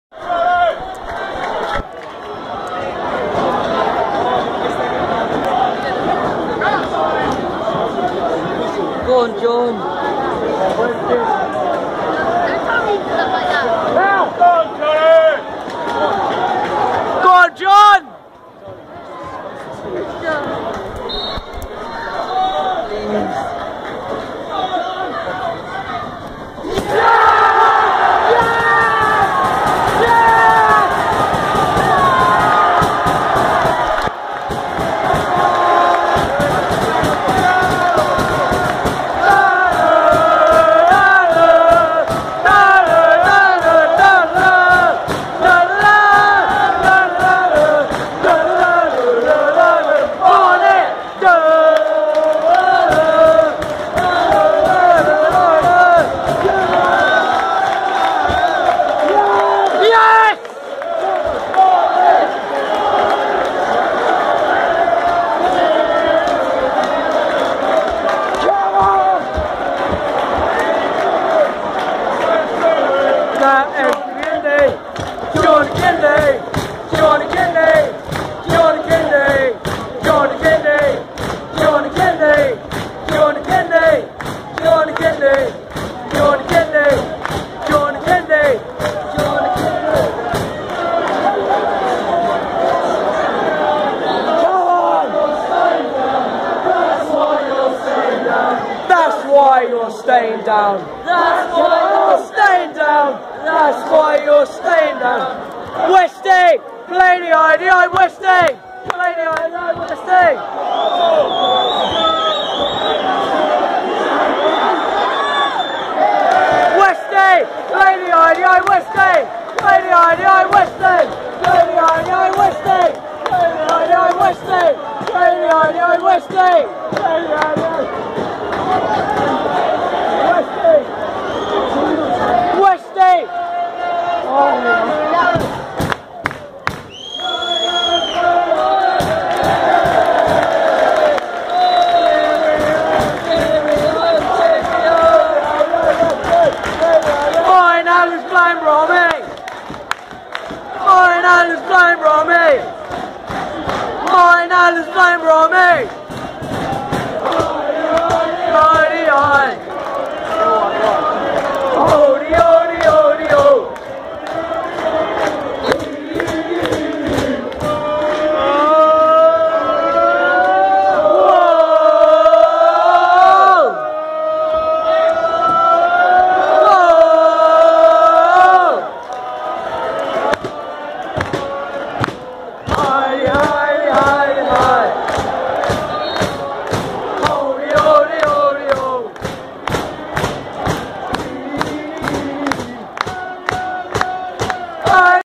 Barnet fans after winner vs luton town
Us lot after a late John Akinde penalty in the last minute final score bees 2-1 luton